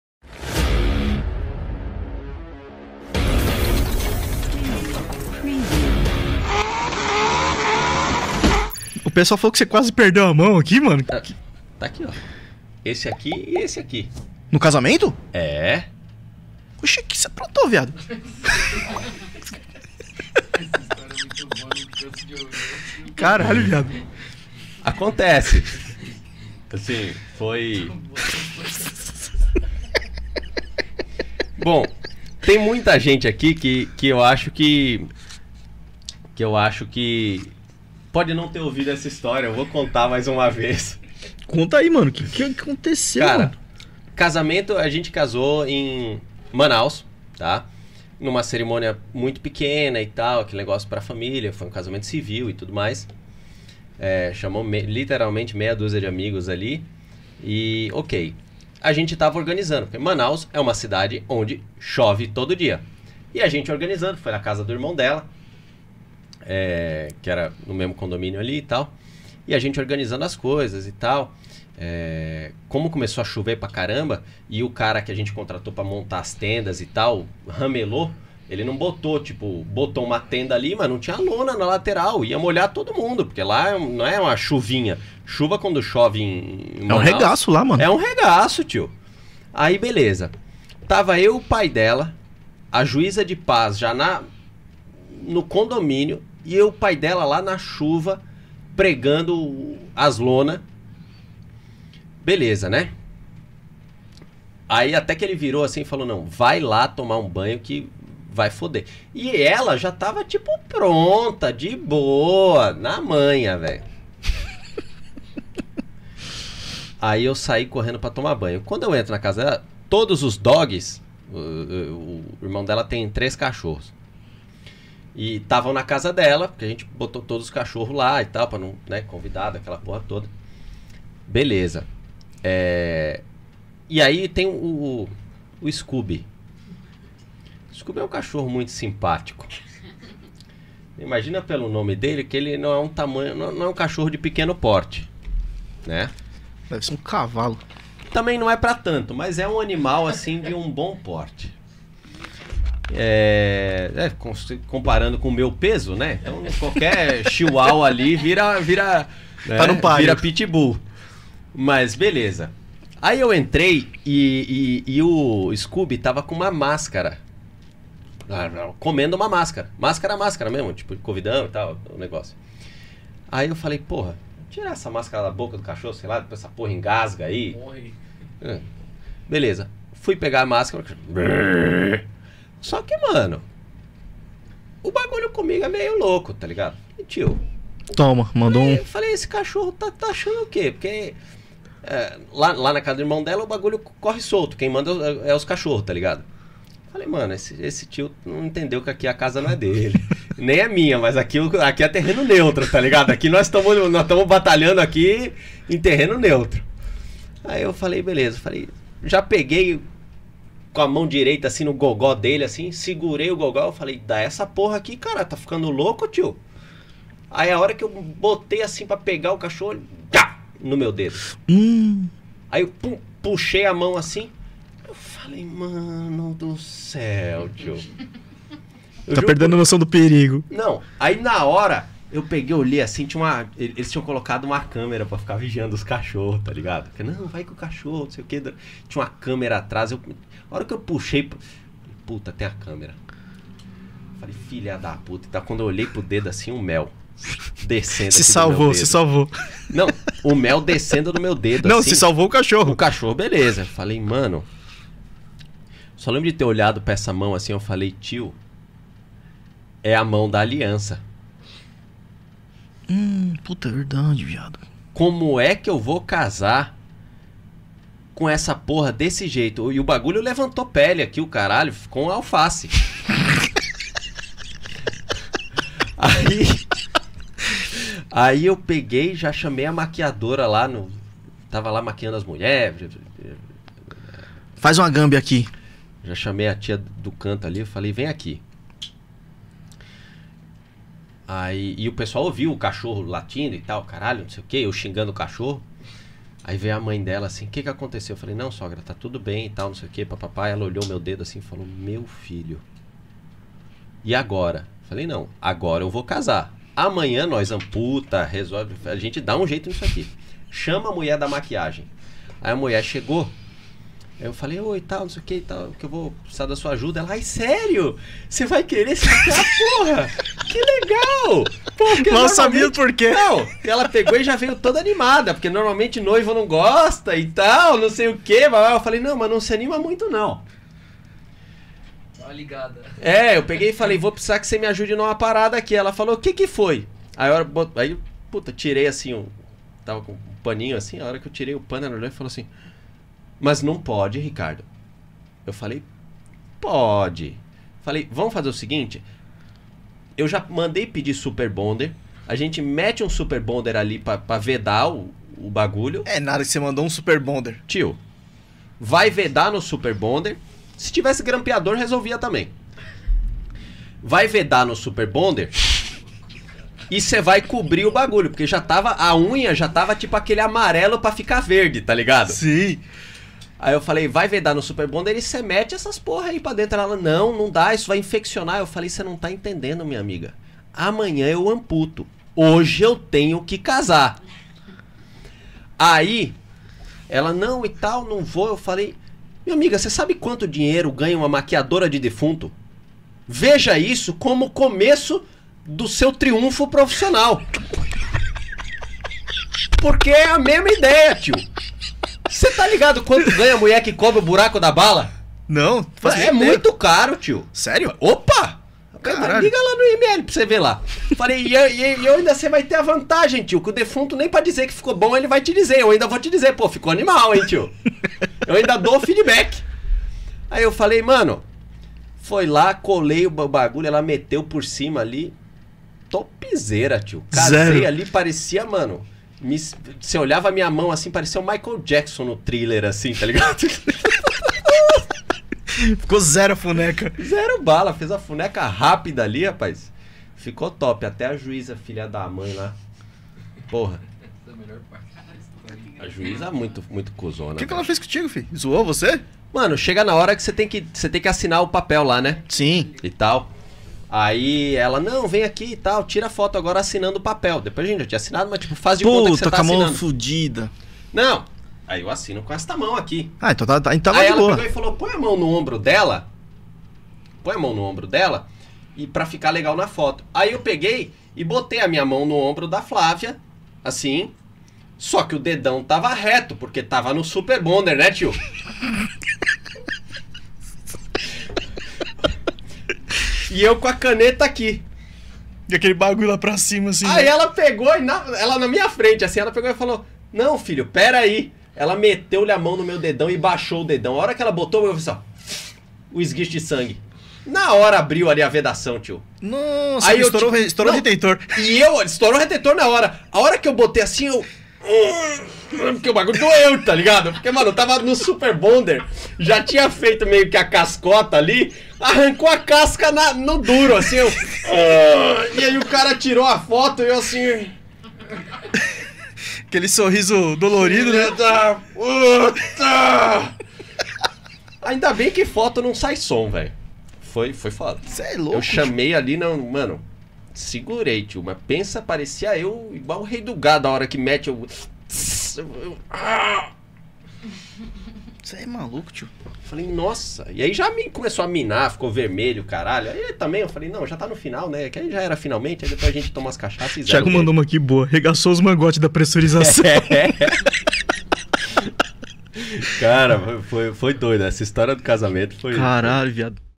RICARDINHO QUASE PERDEU A MÃO NO DIA DO CASAMENTO - TUNERCAST - PIXEL - P9 Power Factory​
TUNERCAST #003 - PIXEL - P9 Power Factory​